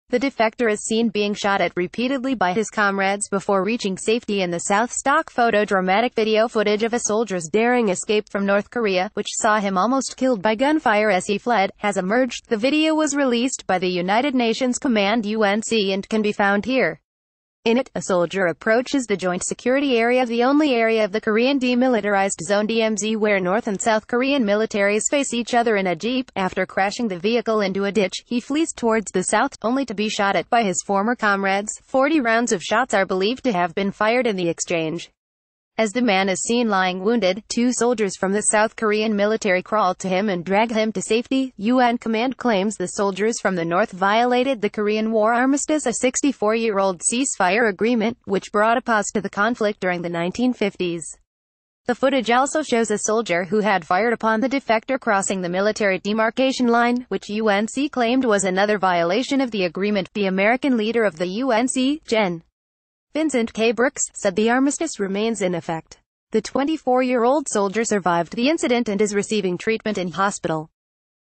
News today-dramatic footage showed North Korean soldier was wounded by gunfire when he fled to Sout
News Today - News today-dramatic footage showed North Korean soldier was wounded by gunfire when he fled to South Korea

The defector is seen being shot at repeatedly by his comrades before reaching safety in the south stock photo Dramatic video footage of a soldiers daring escape from North Korea, which saw him almost killed by gunfire as he fled, has emerged .  The video was released by the United Nations Command UNC and can be found  here. In it, a soldier approachs the Joint Security Area the only area of the Korean Demilitarised Zone DMZ where North and South Korean militaries face each other in a jeep.  Afte...